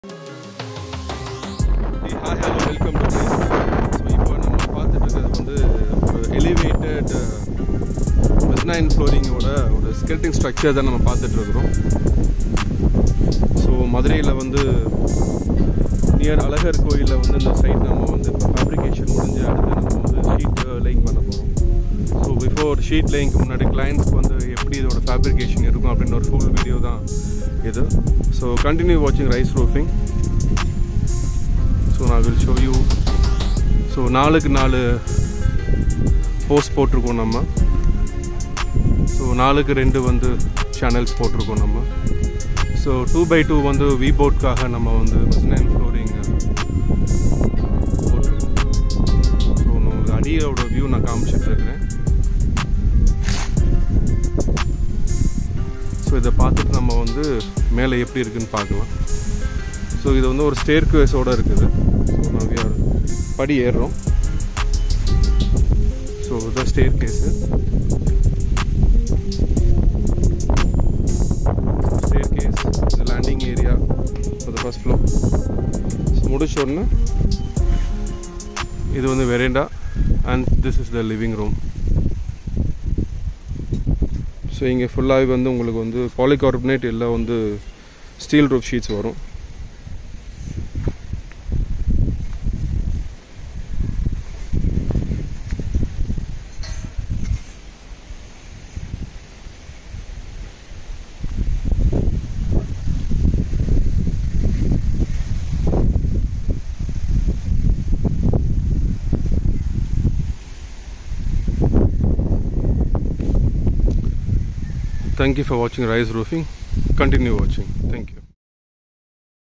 Mezzanine Floor Roofing Work. Near the Mountain.
we are showing the full how to make video of roofing with mezzanine flooring.